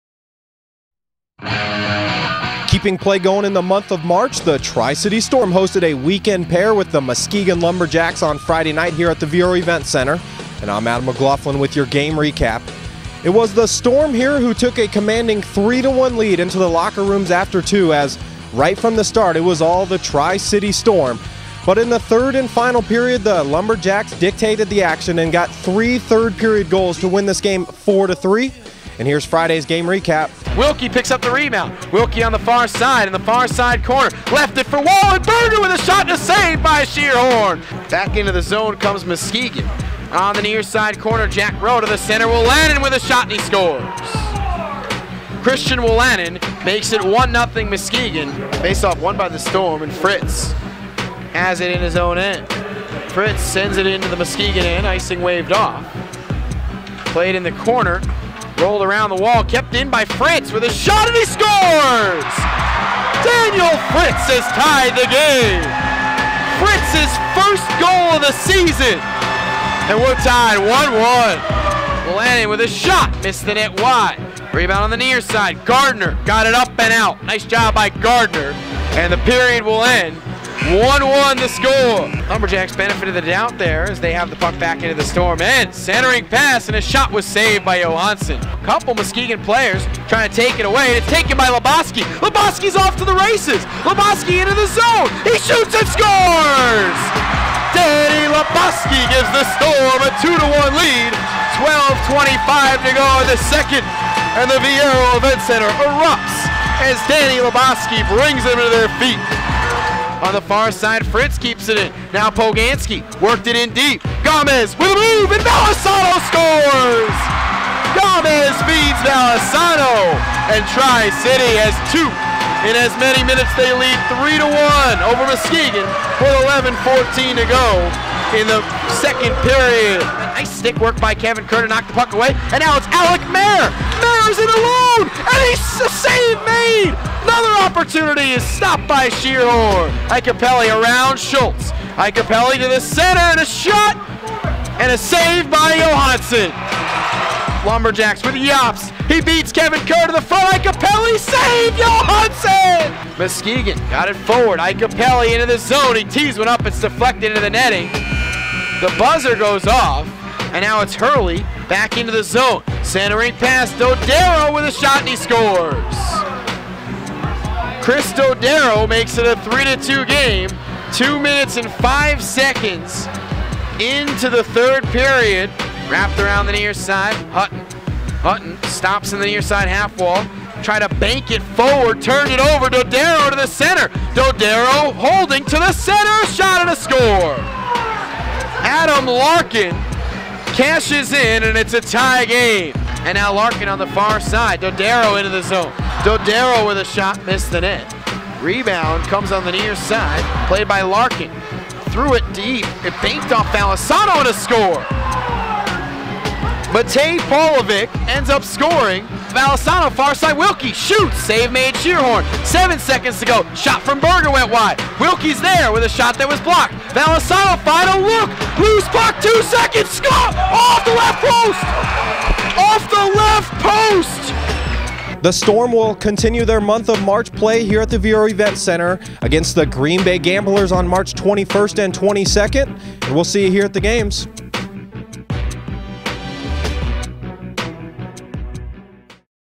March 7, 2014 - Lumberjacks vs. Storm
The Muskegon Lumberjacks (23-22-4) used a three-goal third period to defeat the Tri-City Storm (18-30-3) by a score of 4-3 in the first of a two-game weekend series on Friday night at the Viaero Event Center.

Storm defenseman Daniel Fritz scored his first USHL goal in his 35th career game, while forwards Dan Labosky and Conner Valesano each netted tallies in the loss for Tri-City.

BelievePurple all season long by following Storm hockey on Facebook, Twitter and Instagram.